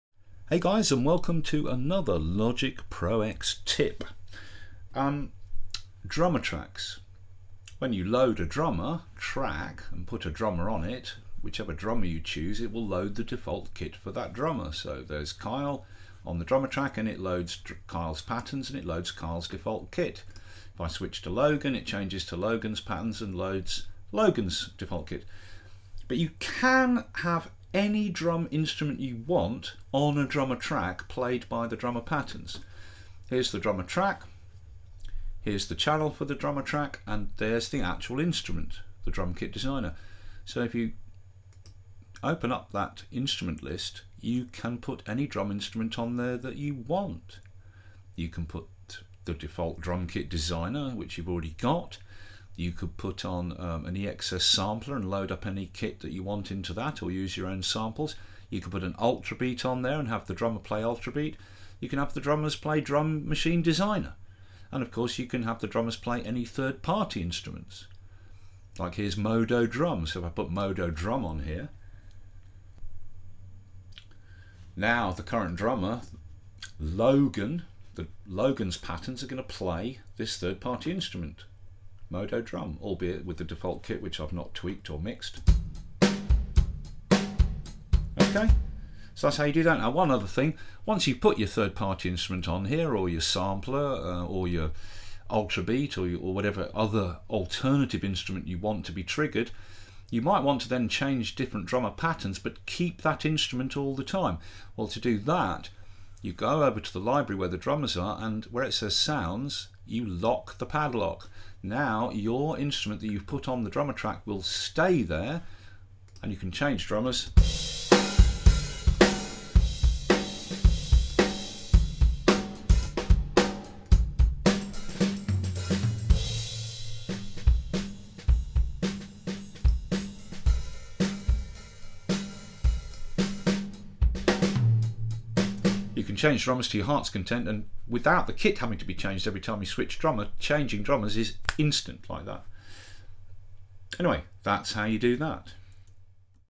Logic Pro X Tips 50 - Drummer Tracks With Any Drum Instrument
You can use Drummer Tracks in Logic with any drum instrument you like, either Logic instruments or 3rd party drum instruments